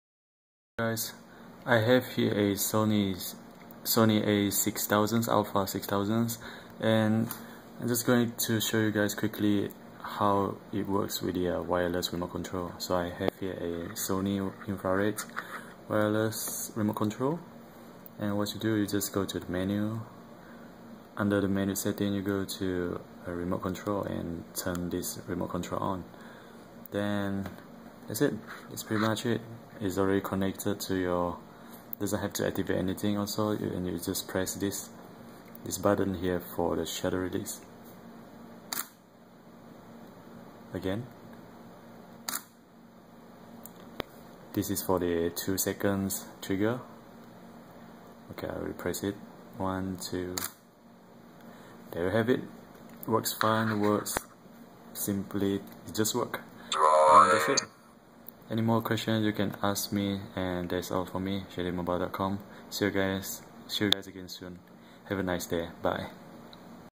Sony Alpha a6000 wireless remote control infrared test
Just testing out the camera wireless remote control trigger. It works fine :)